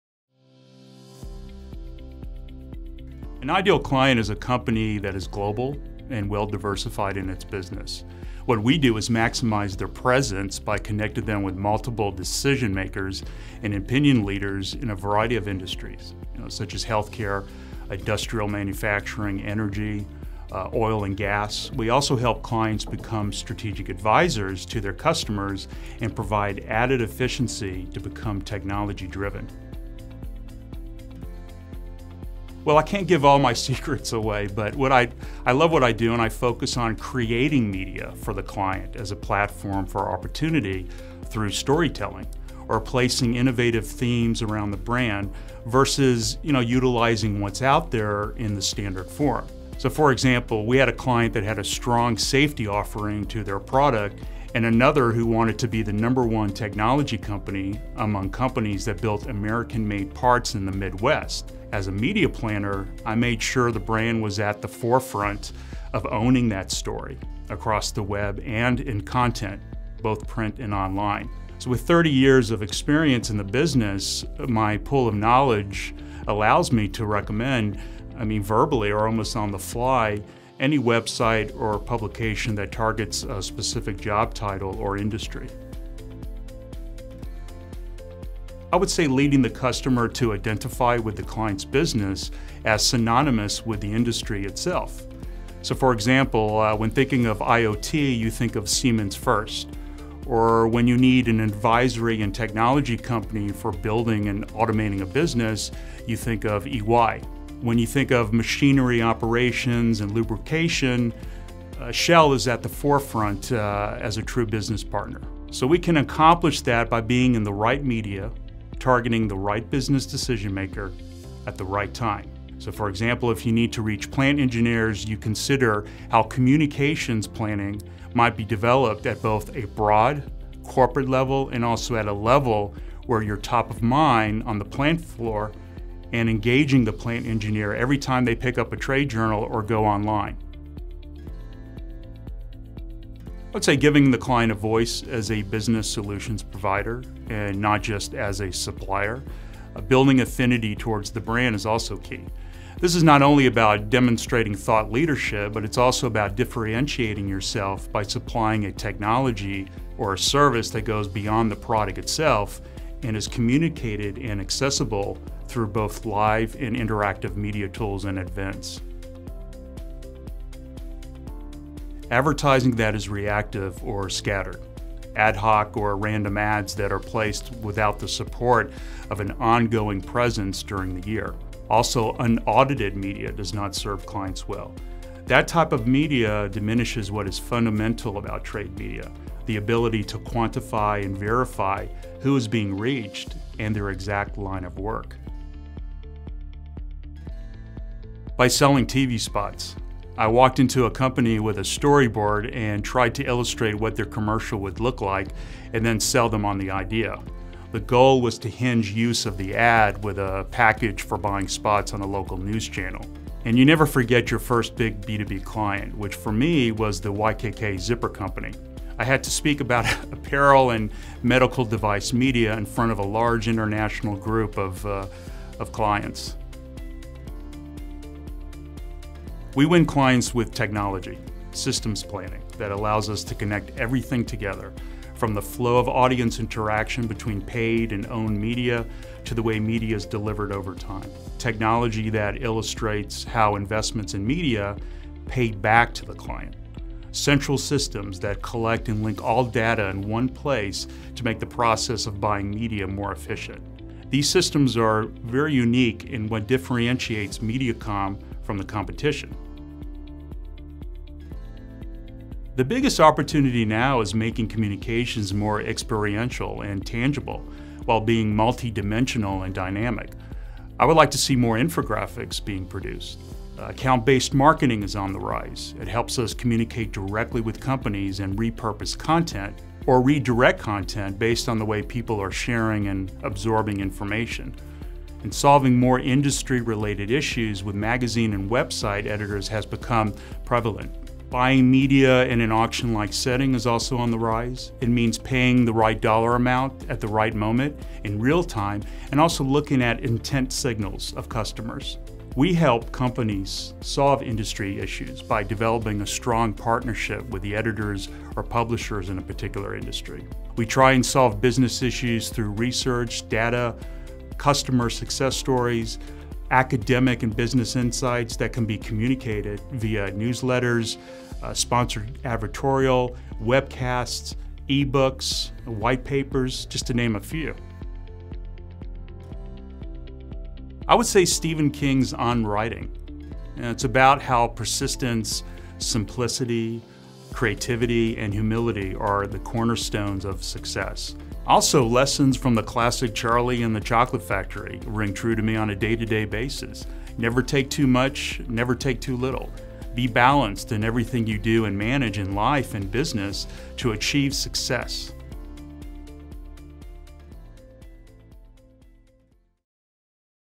Marketing Communications Tips by B2B Media Specialist Kevin Arsham from MEDIACOM
- Who is your ideal client?
- An ideal client is a company that is global, and well diversified in its business. What we do is maximize their presence by connecting them with multiple decision makers and opinion leaders in a variety of industries, such as healthcare, industrial manufacturing, energy, oil and gas. We also help clients become strategic advisors to their customers and provide added efficiency to become technology driven.

- What's your 'Secret Sauce'?
- Well I can't give all my secrets away, but I love what I do and I focus on creating media for the client as a platform for opportunity through story telling. Or placing innovative themes around the brand versus utilizing what's out there in the standard forum.

So, for example, we had a client that had a strong safety offering to their product and another who wanted to be the number one technology company among companies that built American-made parts in the midwest. As a media planner, I made sure the brand was at the forefront of owning that story across the web and in content, both print and online.

So with 30 years of experience in the business, my pool of knowledge allows me to recommend, I mean verbally or almost on the fly, any website or publication that targets a specific job title or industry.

- What results do you get for your clients?
- I would say leading the customer to identify with the client's business as synonymous with the industry itself. So, for example, when thinking of IOT, you think of Siemens first. Or when you need an advisory in technology company for building and automating a business, you think of EY. When you think of machinery operations and lubrication, Shell is at the forefront as a true business partner.
So we can accomplish that by being in the right media, targeting the right business decision maker, at the right time.

So, for example, if you need to reach plant engineers, you consider how communications planning might be developed at both a broad, corporate level and also at a level where your top of mind on the plant floor and engaging the plant engineer every time they pick up a trade journal or go online.

- What strategy works best for your clients?
- I'd say giving the client a voice as a business solutions provider and not just as a supplier. A building affinity towards the brand is also key. This is not only about demonstrating thought leadership but it's also about differentiating yourself by supplying a technology or a service that goes beyond the product itself and is communicated and accessible through both live and interactive media tools and events.

- What does NOT work well?
- Advertising that is reactive or scattered.
Ad hoc or random ads that are placed without the support of an ongoing presence during the year. Also, unaudited media does not serve clients well. That type of media diminishes what is fundamental about trade media, the ability to quantify and verify who is being reached and their exact line of work.

- How did you get your 1st client?
- By selling TV spots. I walked into a company with a storyboard and tried to illustrate what their commercial would look like and then sell them on the idea. The goal was to hinge use of the ad with a package for buying spots on the local news channel. And you never forget your best big B2B client, which, for me, was the YKK zipper company. I had to speak about apparel and medical device media in front of a large international group of clients.

- How do you win clients now?
- We win clients with technology, systems planning that allows us to connect everything together from the flow of audience interaction between paid and owned media, to the way medias delivered over time. Technology that illustrates how investments in media pay back to the client. Central systems that collect and link all data in one place to make the process of buying media more efficient. These systems are very unique in what differentiates MediaCom from the competition.